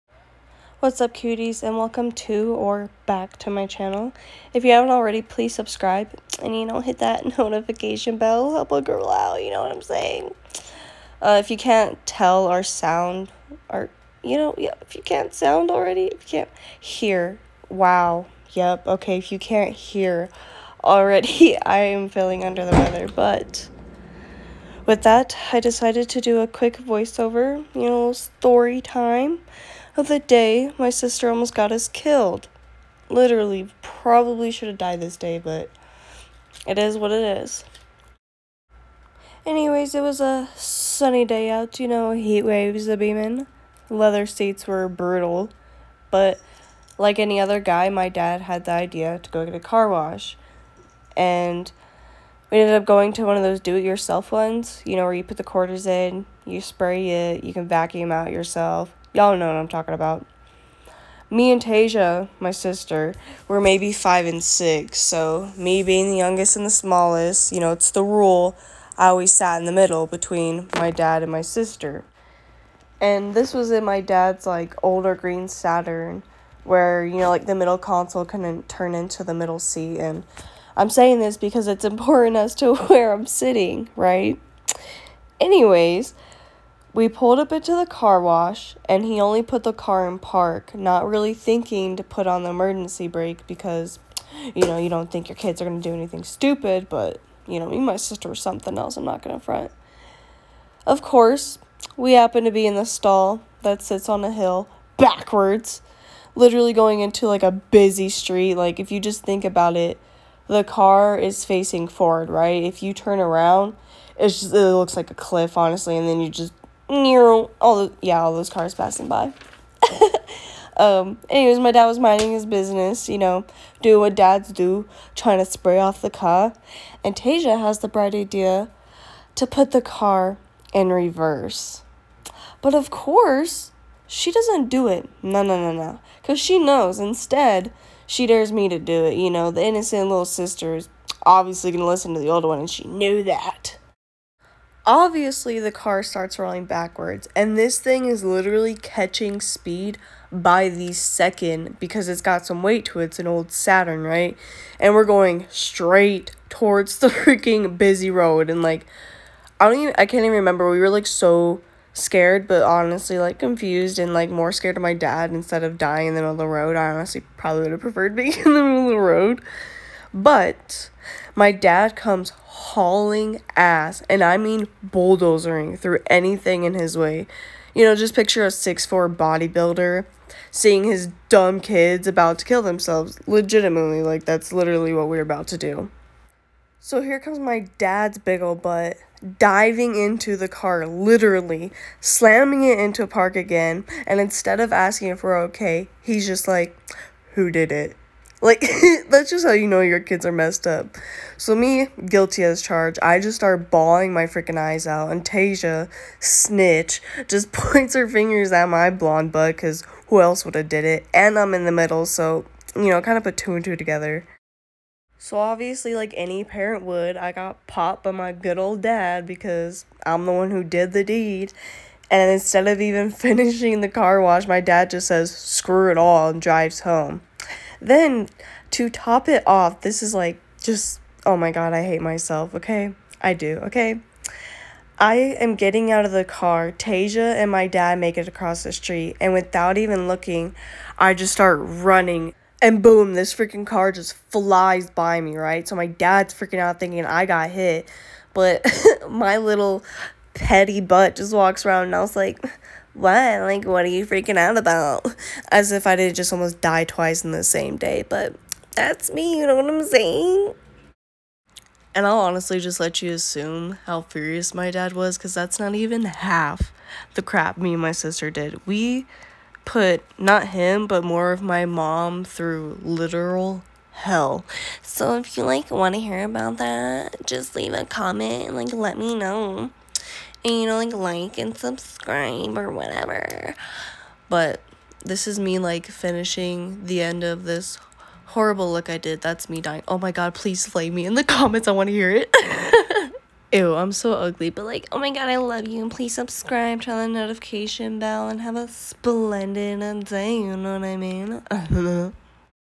Voiceover Storytime? The Day My Sister Almost Got Us Killed!!!
Granted yet another Monday!! I hope you all are staying warm and safe!! I’m back again with yet another short Storytime during a look that went south🤣. Please leave your reactions in the comments and other makeup ideas you want to see me do!! I love hearing them. 🛎 be an A1 Day1 !!